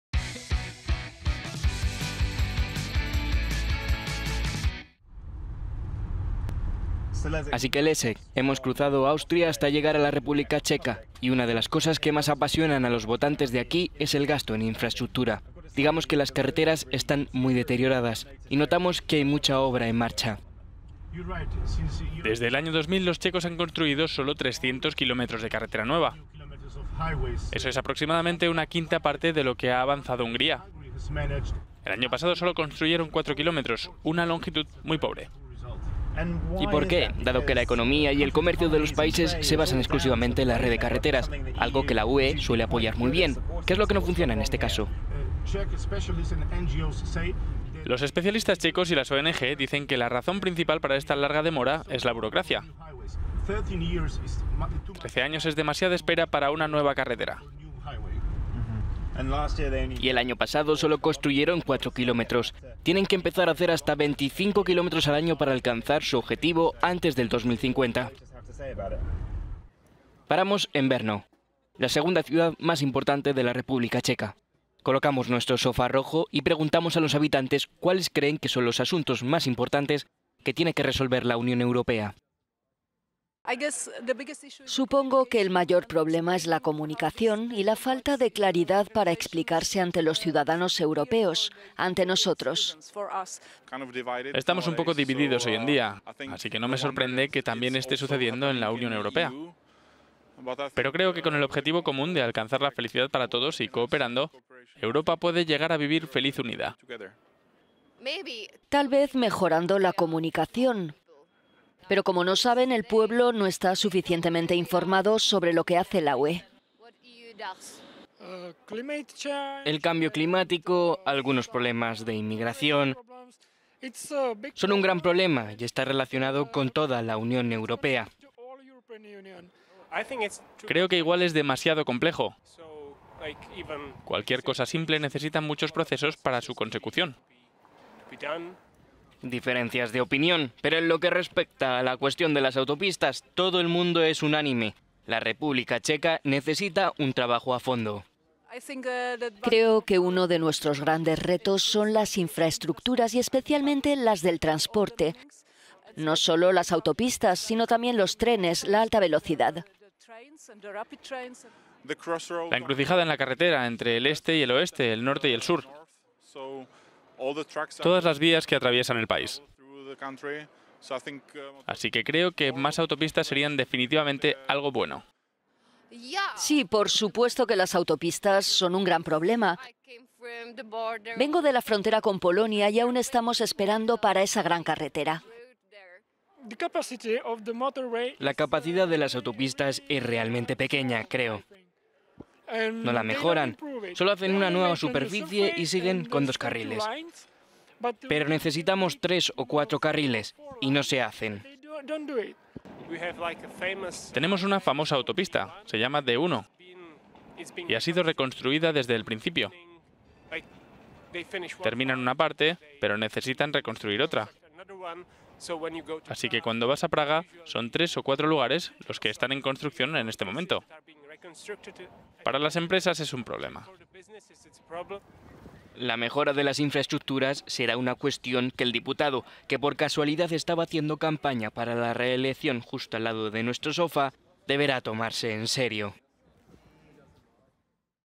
Euroadtrip En ruta a las europeas-Día 43: carreteras checas a paso de tortuga
Desde el año 2000, los checos han construido sólo 300 km de carreteras nuevas. Eso es aproximadamente una quinta parte de lo que ha avanzado Hungría. El año pasado sólo construyeron 4 km.…